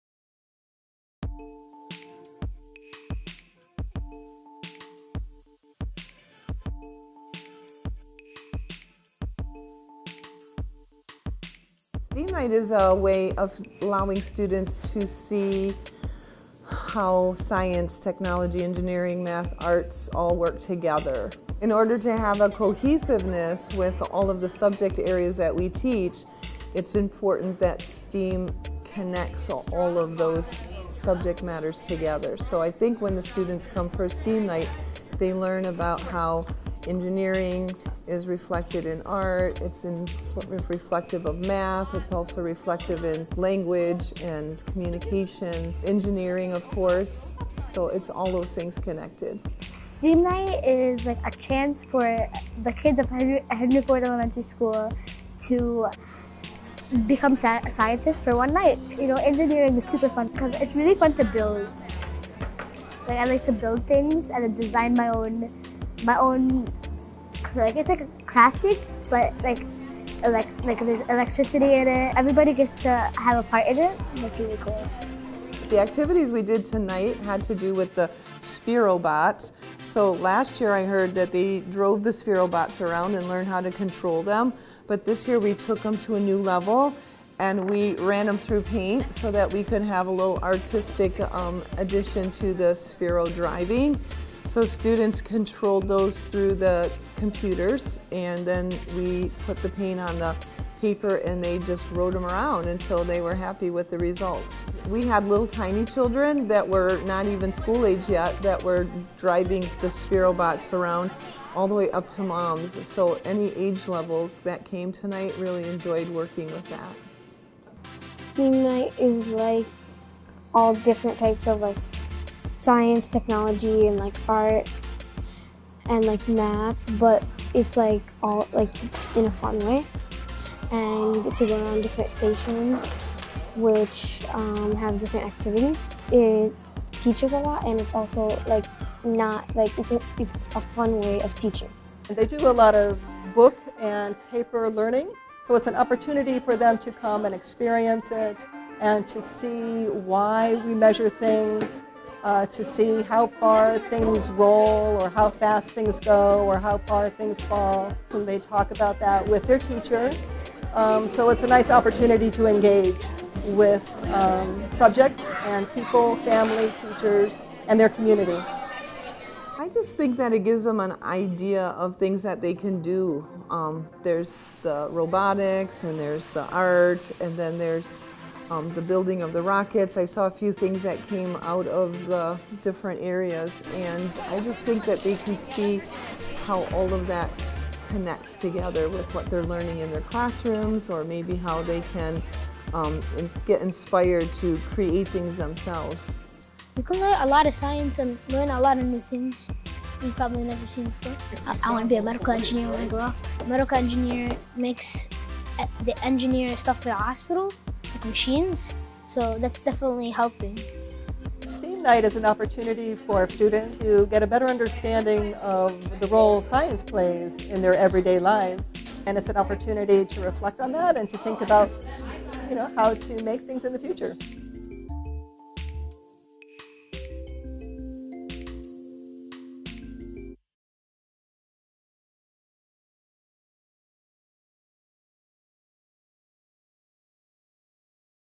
STEAM Night 2020 | Henry Ford Elementary | Feb. 11th, 2020
Henry Ford Elementary hosted their annual STEAM night in February. Student were able to experience a number of hands on activities that focused on science, technology, engineering, art, and math.